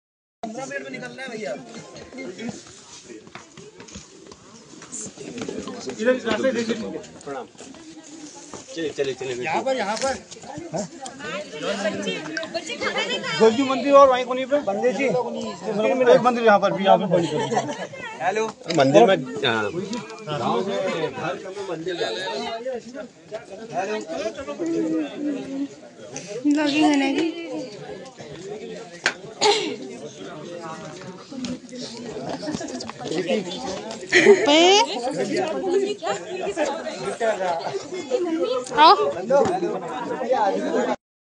Ms Dhoni pauchai apnai Ghar Almora JAYTI GAO mai Dekhiay Nazara kya hua?
After former Indian Cricket team captain announced retirement from International cricket on Saturday, people of his family's native village Lwali and elsewhere in Uttarakhand appealed him to return to his native state and help youth battle unemployment.

Hayat Singh, his distant cousin who wants a stadium in his village on his younger brother's name recalling his visits to Lwali village said, "We all know him as our own 'Mahi'. He used to play long helicopter shots then also. His shots were so long that usually we could not find the ball. Now he will have time so we urge him to do something for the youth of the village and Uttarakhand."

His relatives and residents of his family's native village also expressed surprise over his sudden retirement. "We are all shocked as hee has more cricket left in him. We think he should have played more," said Dinesh Dhoni, the village pradhan.

Recalling his last visit in winters of 2004 in a relative's wedding in Lwali, Hayat Singh added, "He was as cool and calm you see him in the television and used to like pahadi food. And of course lots of cricket and helicopter shots. We have had never seen anyone in our local tournaments like that."